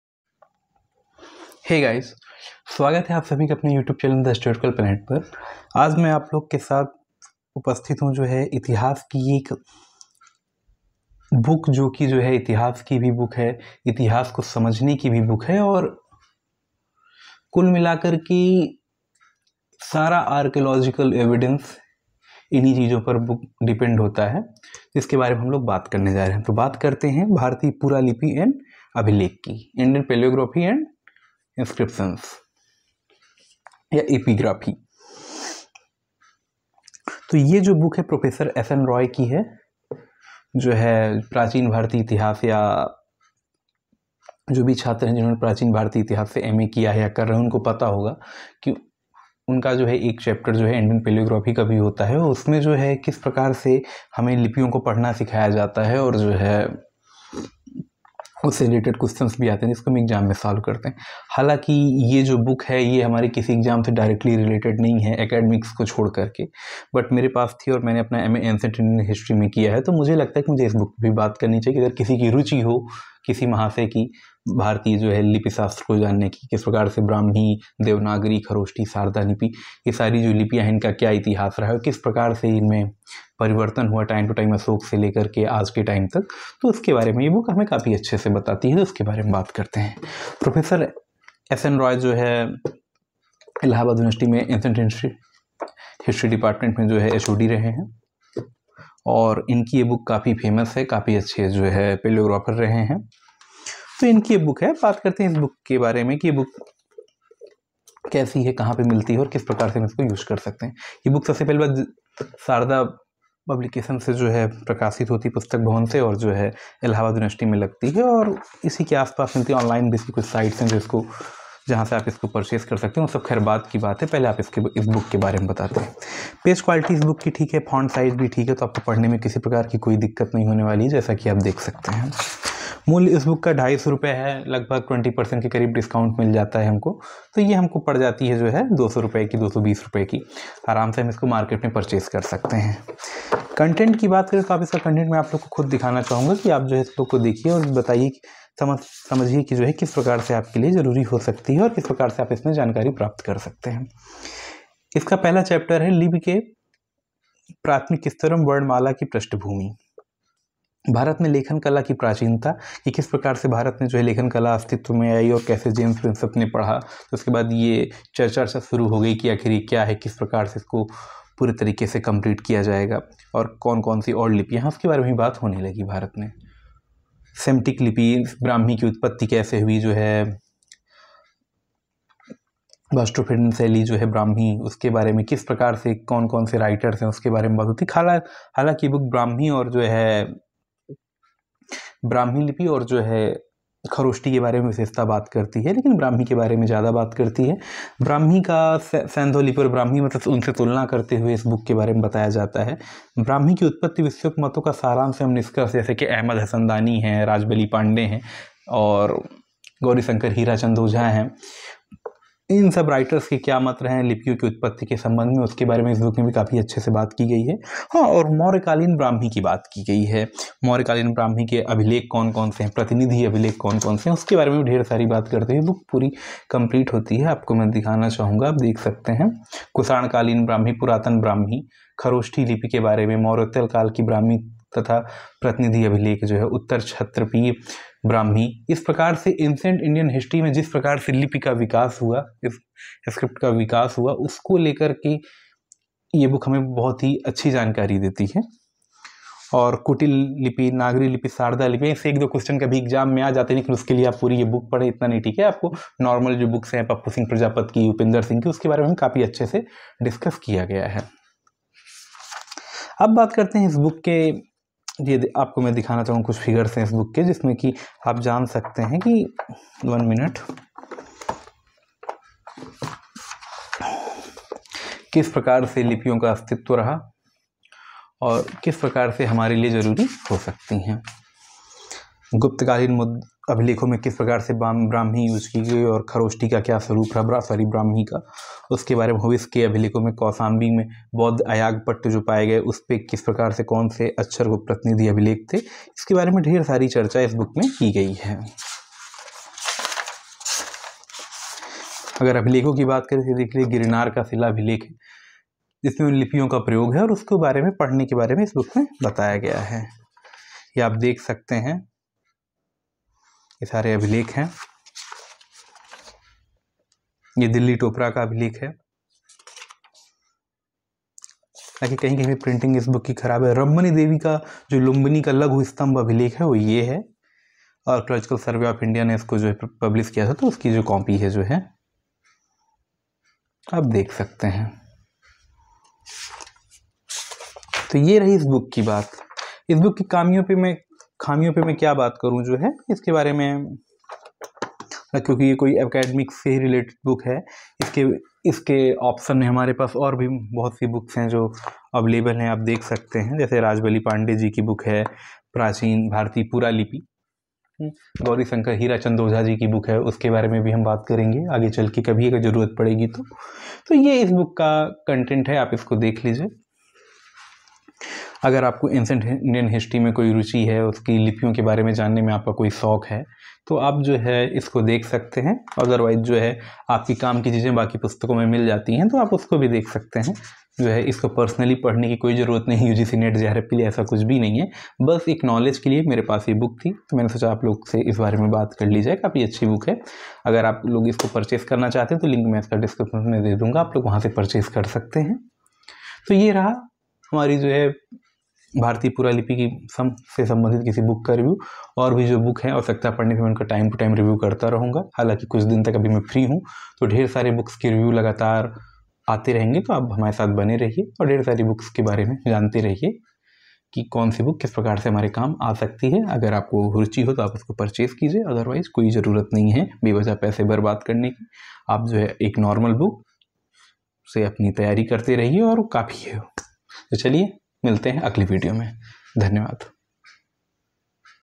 BOOK REVIEW : INDIAN PALAEOGRAPHY & EPIGRAPHY BY S.N. ROY |FOR MA , BA AND RESEARCH IN HISTORY
प्राचीन भारतीय अभिलेख और उनकी लिपियां को समझने के लिए एक उपयोगी पुस्तक जिसके माध्यम से हम  उन सभी अभिलेखो को समझ सकते हैं जिसके द्वारा हमारे प्राचीन भारतीय इतिहास का अध्ययन किया जाता है।